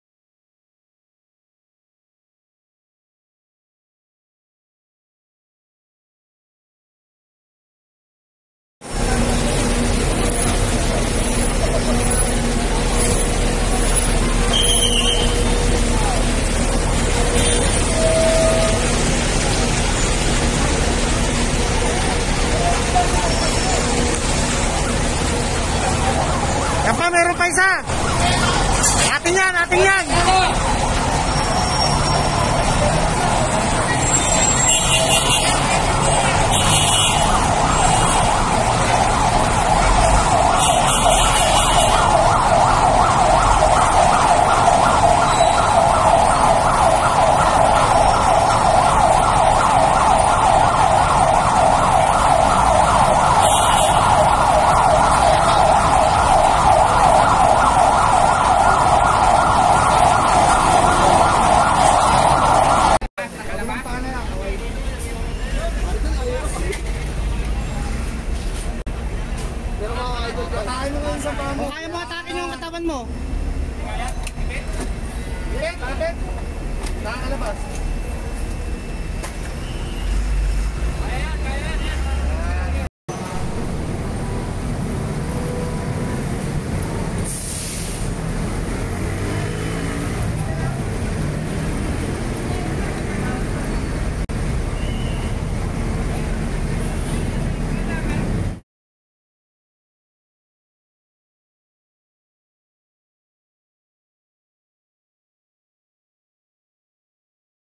Bus, ambulance figure in accident on Edsa-Shaw tunnel
A bus and an ambulance figured in an accident along the southbound lane of the Edsa-Shaw Boulevard tunnel on Friday night, the Metropolitan Manila Development Authority (MMDA) said.